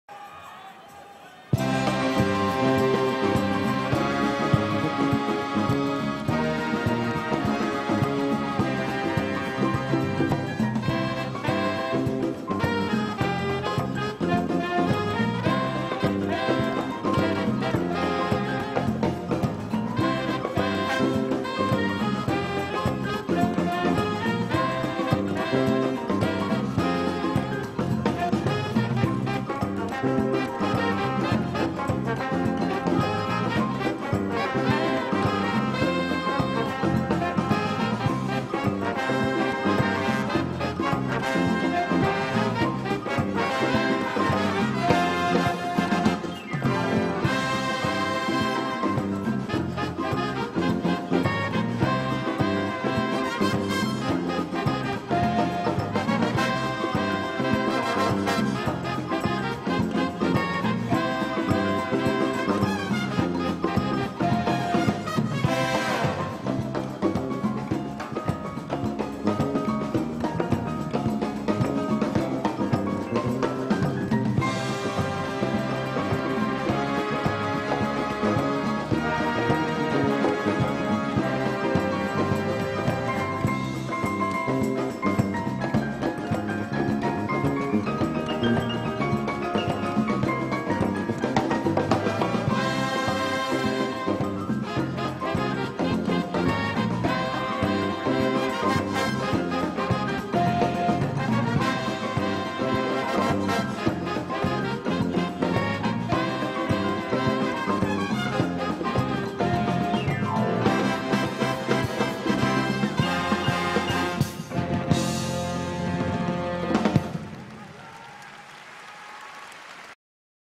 Slavik Kryklyvyy and Karina Smirnoff Dance Samba
Two greats performing a lively samba.